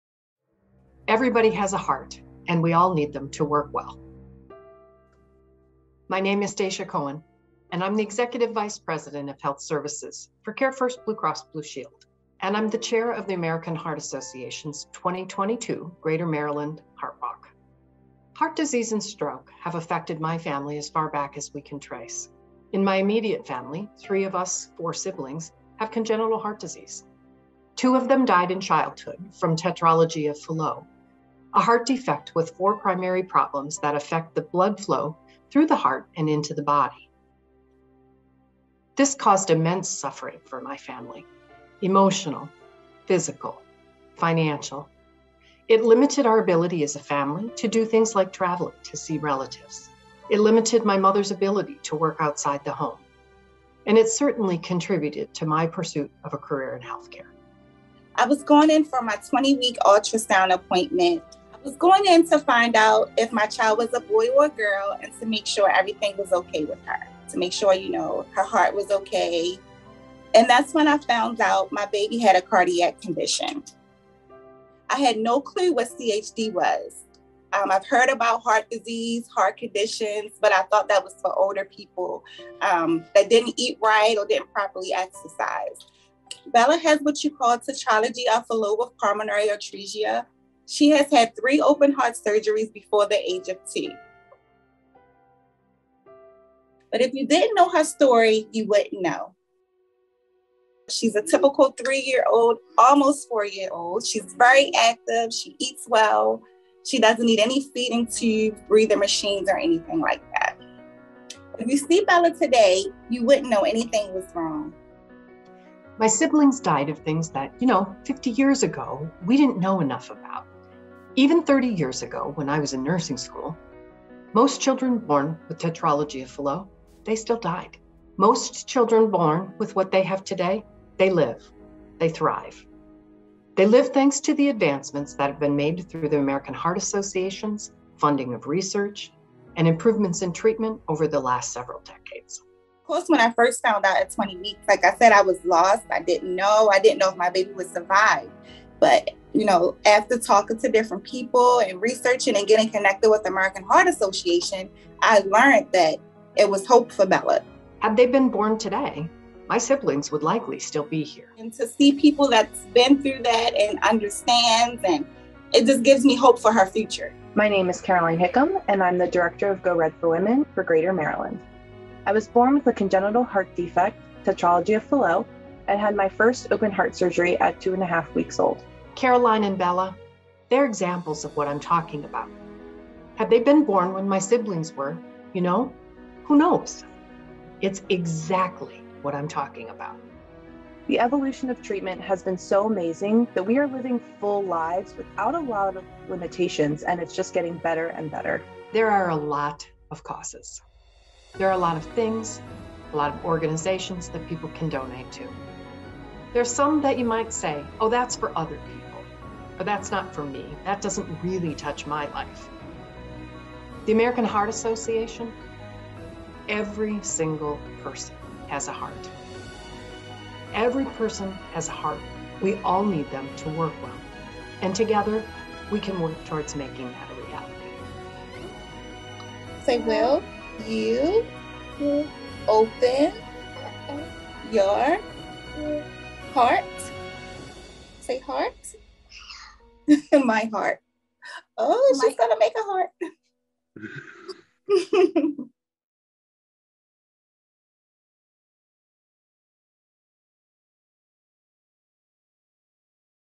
Everyone Has a Heart: A Story of Congenital Heart Defects
CareFirst BlueCross Blue Shield Executive Stacia Cohen, Greater Maryland Go Red Director Caroline Hickam and mom Latisha Wilborne and daughter Bella share their stories about how Congenital Heart Defects, particularly Tetralogy of Fallot, have touched their lives.